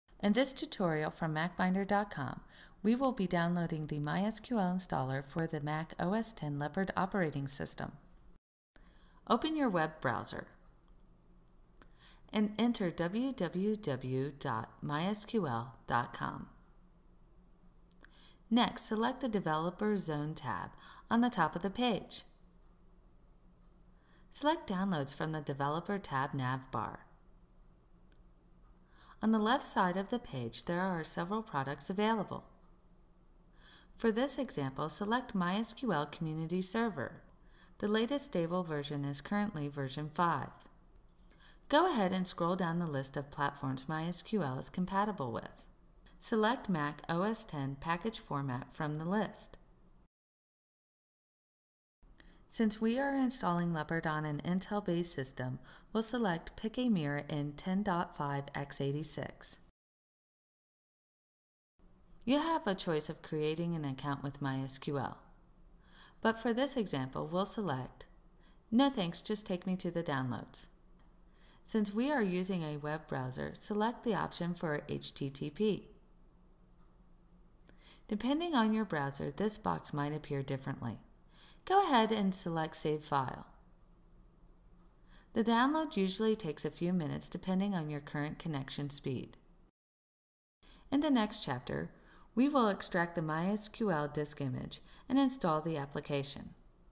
Install MySQL on OS X 10.5 Leopard
How and where to download the MySQL package for Apple OS X 10.5 Leopard